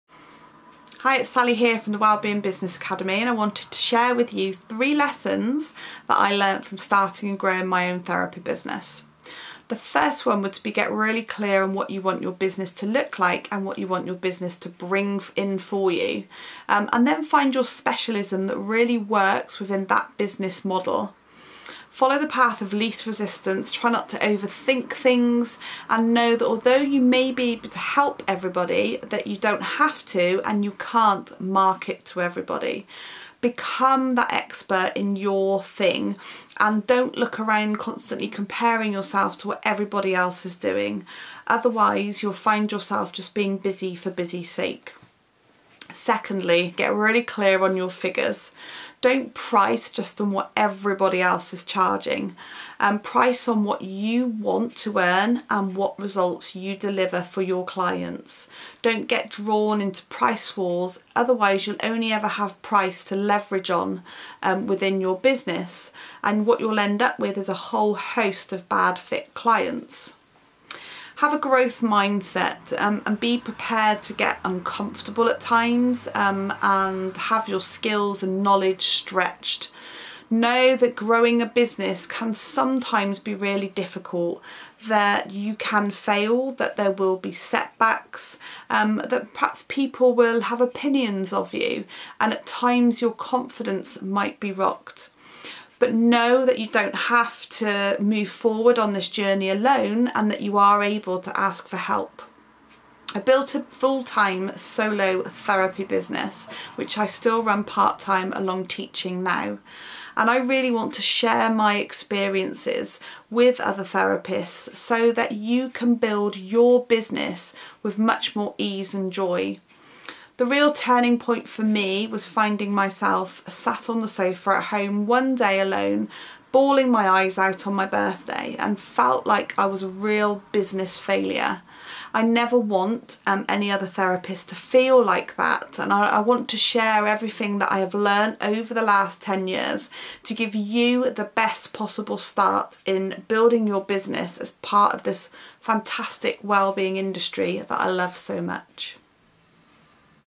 Three lessons learnt when starting a business
We share 3 key things to be clear on when starting a brand new business.